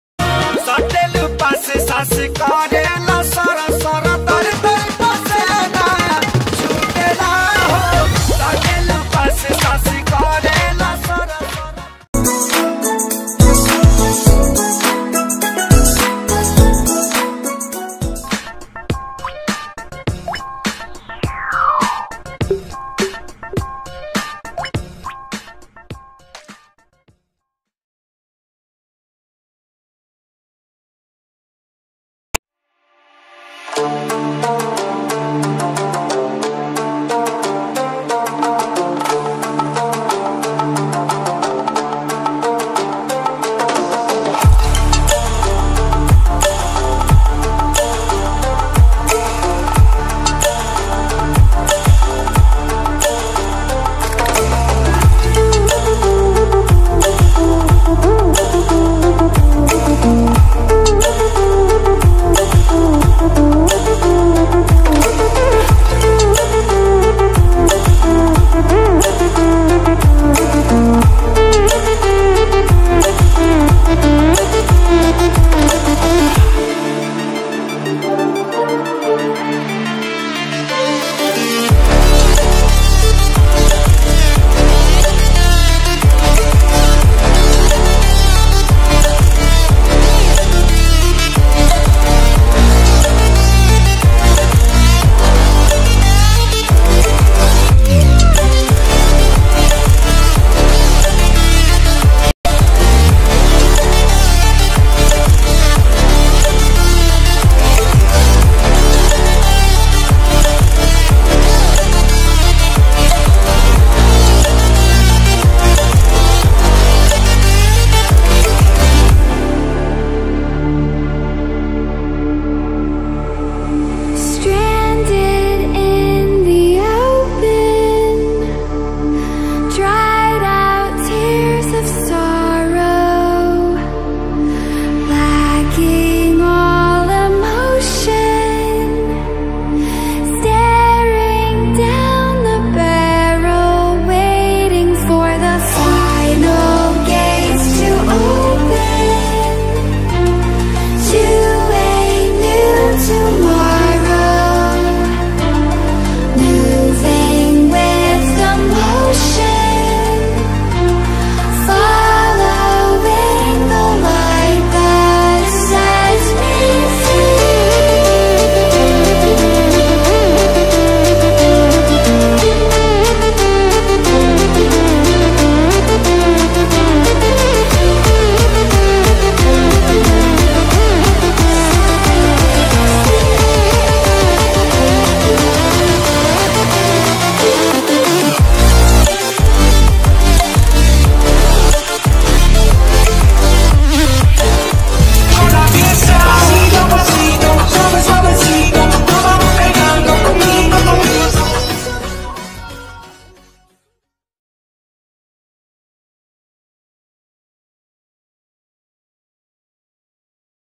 Mirror Art in picsart
VIEWERS TODAY AM SHOW U HOW TO EDIT YOUR IMAGE LIKE A PROFESSIONALLY  IN YOUR MOBILE JUST FINISH YOUR EDITING AM SURE WILL SMILE IN YOUR FACE

Whtsapp group-

picsart editing,
picsart best editing,
picsart tutorial,
picsart,
picsart app,
picsart android,
picsart all,
picsart animation,